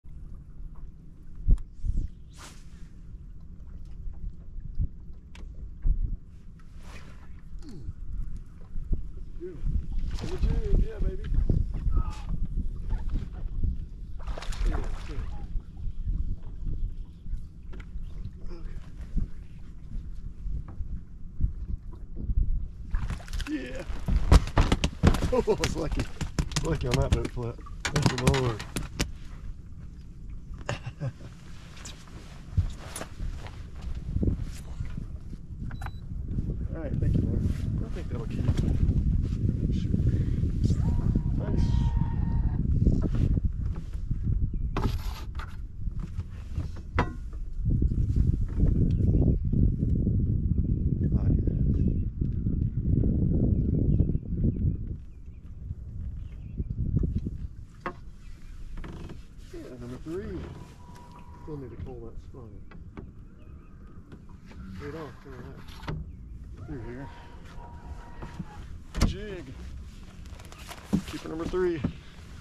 Dramatic Boat Flip in NTX Bass Elite Series Tourney on Texoma
Lake Texoma
NTX Bass Elite Series Stop #3